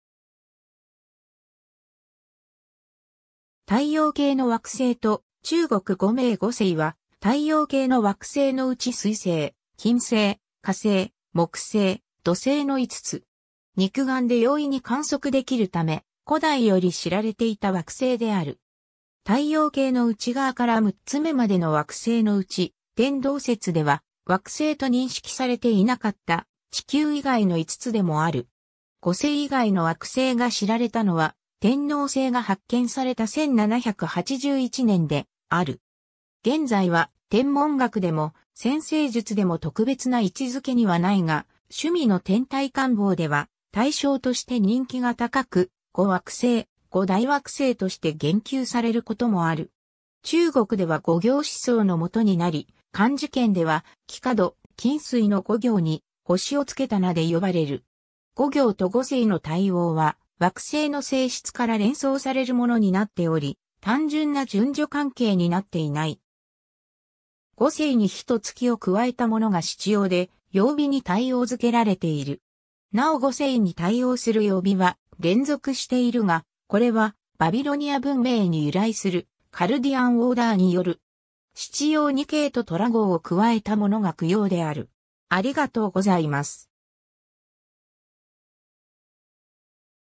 五星
太陽系の惑星と中国語名（繁体字） 五星（ごせい）は、太陽系の惑星のうち水星・金星・火星・木星・土星の5つ。
 肉眼で容易に観測できるため、古代より知られていた惑星である。
太陽系の内側から6つめまでの惑星のうち、天動説では惑星と認識されていなかった地球以外の5つでもある。
五星（と地球）以外の惑星が知られたのは、天王星が発見された1781年である。
 現在は天文学でも占星術でも特別な位置づけにはないが、趣味の天体観望では対象として人気が高く、五惑星・五大惑星として言及されることもある。
 中国では五行思想の元になり、漢字圏（中国語、日本語、朝鮮語、ベトナム語）では「木火土金水」の五行に「星」をつけた名で呼ばれる（ただしベトナム語では「星○」の語順）。
五行と五星の対応は、惑星の性質（わかっていたのは運行・色・光度程度だが）から連想されるものになっており、単純な順序関係になっていない。
 五星に日と月を加えたものが七曜で、曜日に対応付けられている。
なお五星に対応する曜日は連続しているが、これはバビロニア文明に由来する「カルディアンオーダー (chaldean order)」による。
七曜に計都と羅睺を加えたものが九曜である。